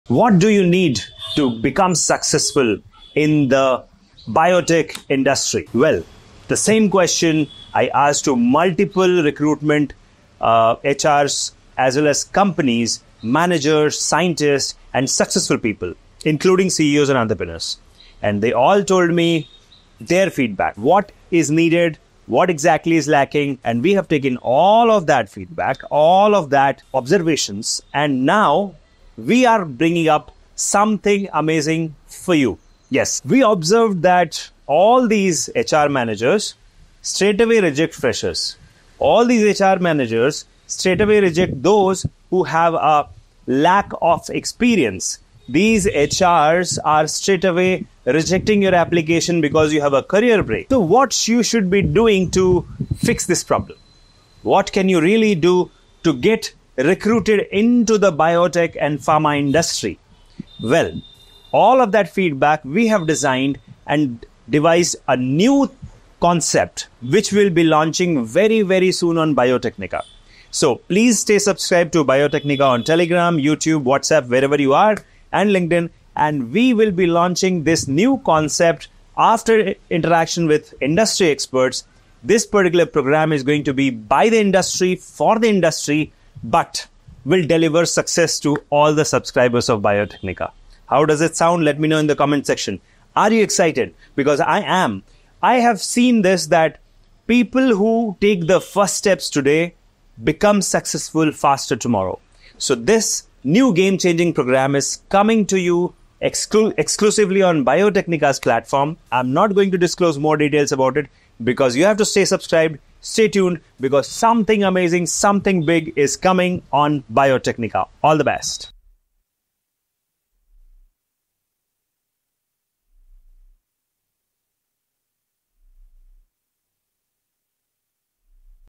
Get Ready To Remove The FRESHERS Tag - Something Grand is Coming at Biotecnika
Are you ready to elevate your career in the biotech industry? Say goodbye to the 'FRESHERS' tag with Biotecnika's groundbreaking new program! 🌟 Join us as we unveil an innovative solution crafted from insights by HR managers and industry leaders. 🚀 Don't miss out on this game-changing opportunity designed to propel your success in biotech and pharma.